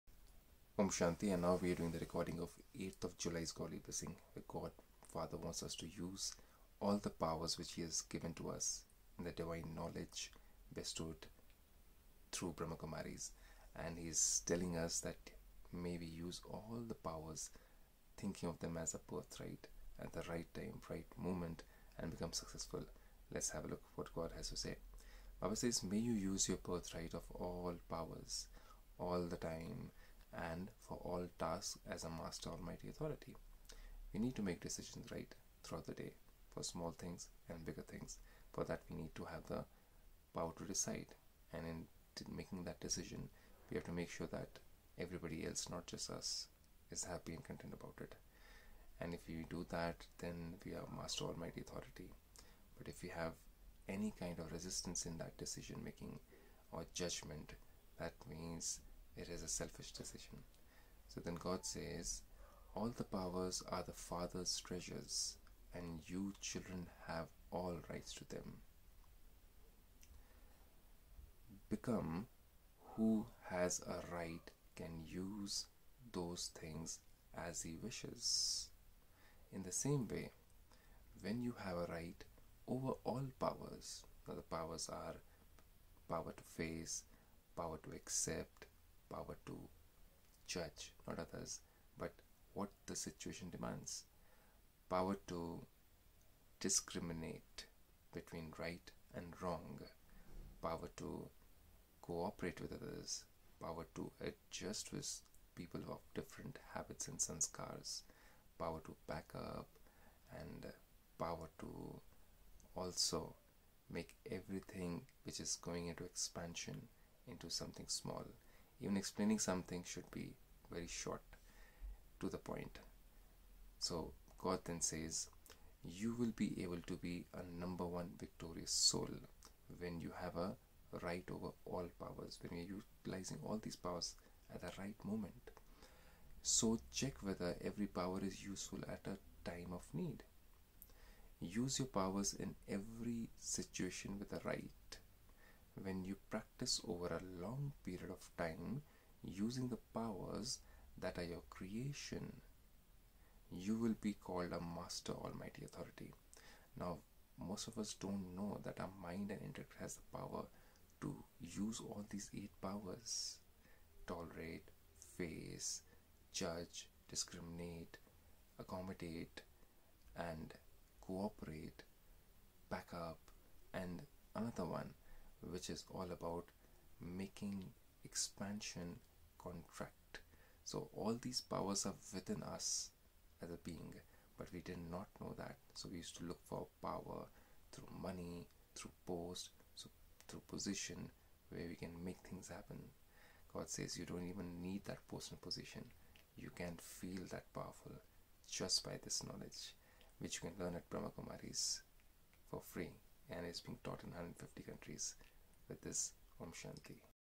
Using the Eight powers received from God's infinite treasure of knowledge as birthright.
Day -158 Using the Eight powers received from God's infinite treasure of knowledge as birthright.

Om Shanti .